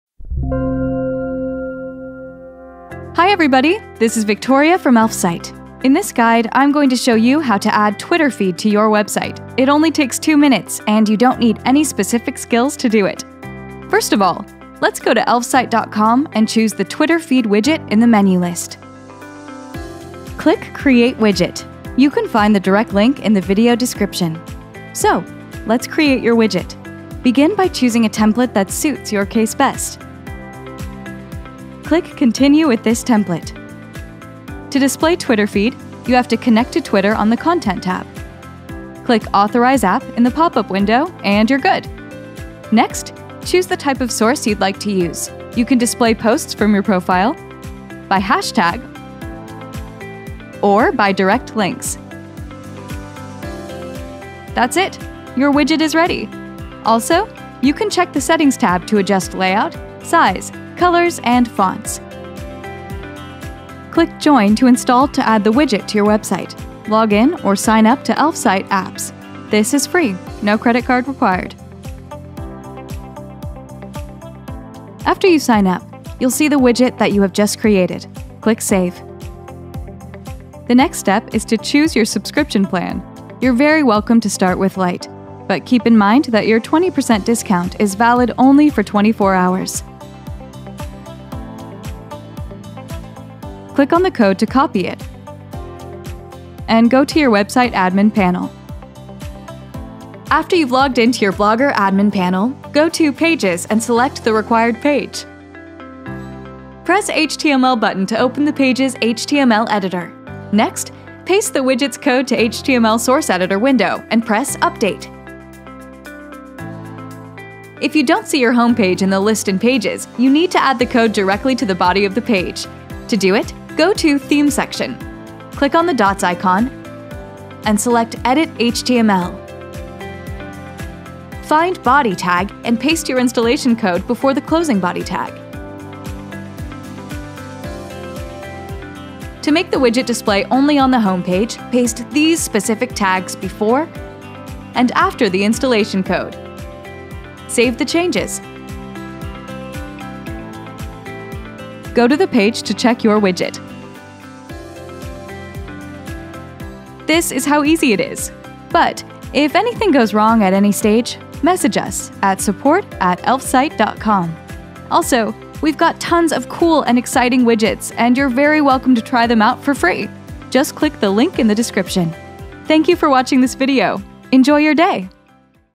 How to Add Twitter Feed to Blogger
Add Twitter Feed to Blogger website in only 2 minutes -

Quick, simple, free, and no-coding.

Advantages:
• Adjustable popups with switchable elements
• Handy and intuitive editor with no coding required
• Three displayable kinds of Twitter content

How to embed Twitter Feed to Blogger website?
To demonstrate the feed on your website, take these three easy actions

1) Set up and customize the widget;
2) Save a unique code from in the notification window;
3) Embed the received code on the website.